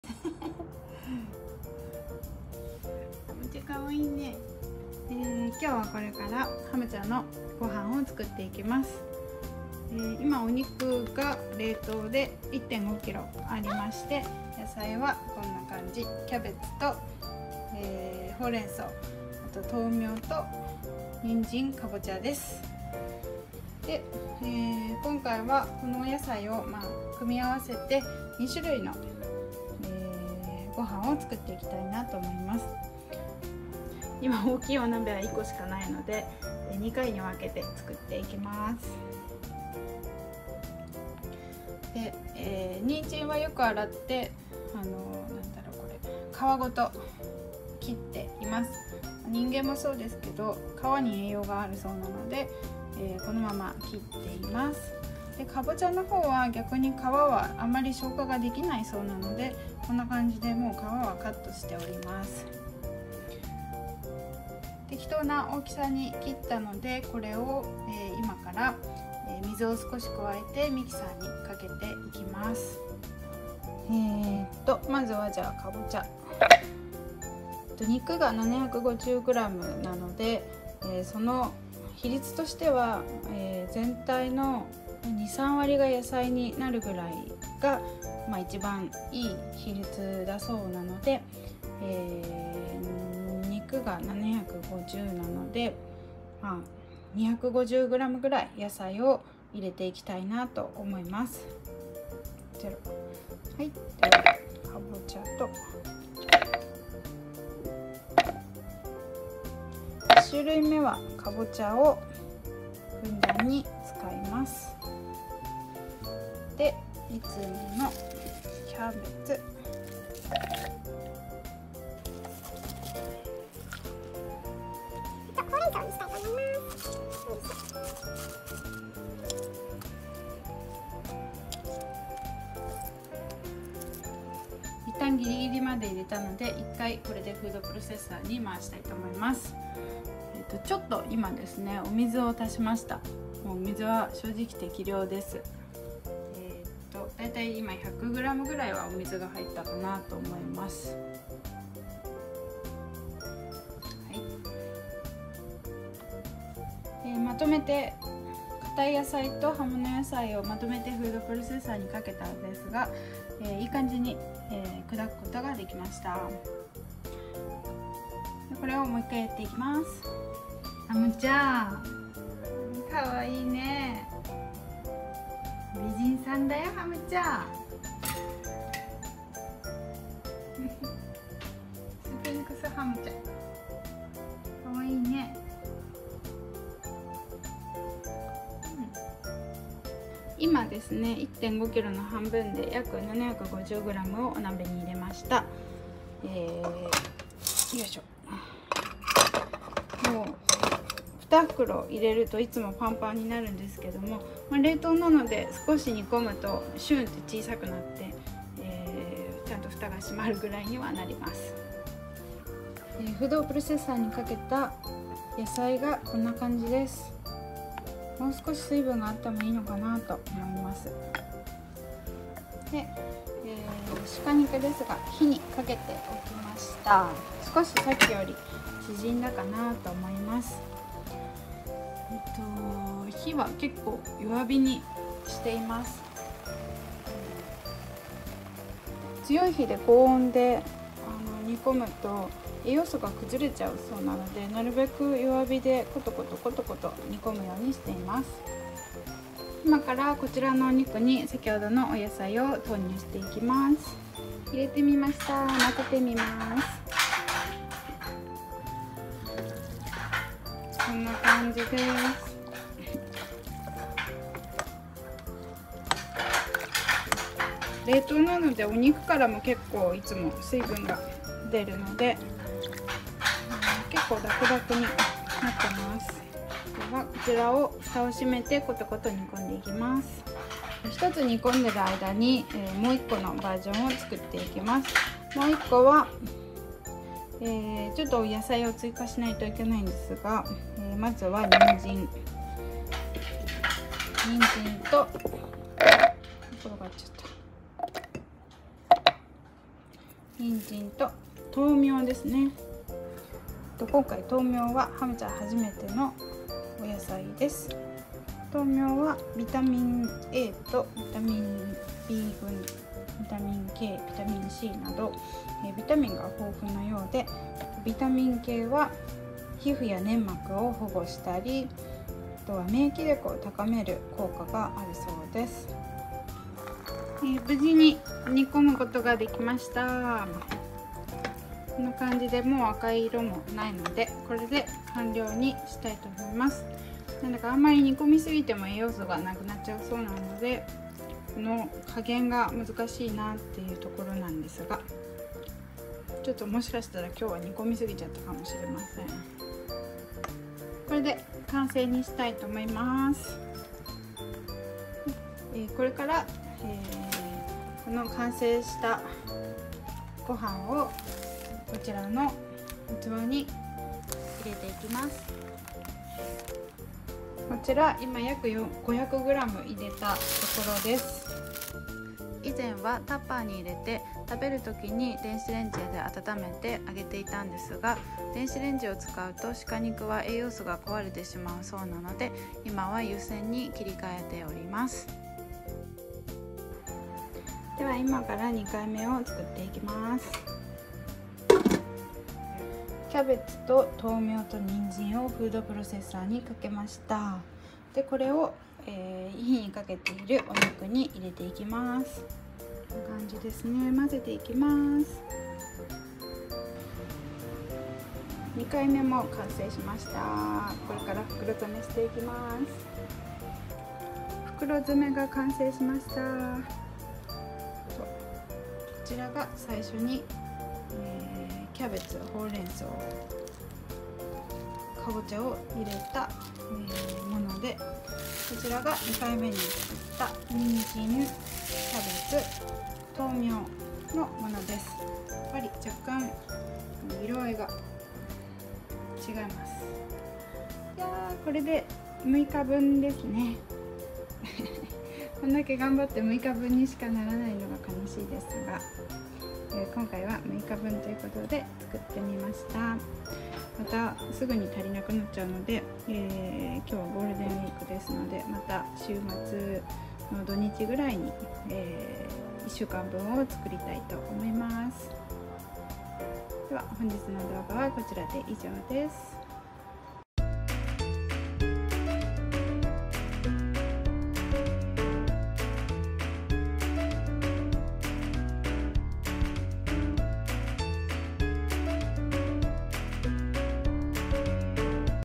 【手作りご飯】アレルギーの多いグレートピレニーズの子犬のための鹿肉と野菜の手作りご飯
2023年5月
アレルギーの多いはむちゃ。
そして犬は本来は肉食動物なので肉食の食事の方が良いということで、はむちゃには生後7ヶ月頃から手作りご飯を混ぜて与えています。
市販のものだとZiwiやK9が肉食で良さそうですが、ラインナップの中ではZiwiの鹿肉しか食べられず、それが450gで1万円程度とものすごく高いので、手作りご飯を作るようになりました。
今回は鹿肉と野菜を煮込んだものを作ったのですが、その様子を動画にまとめてみました☺︎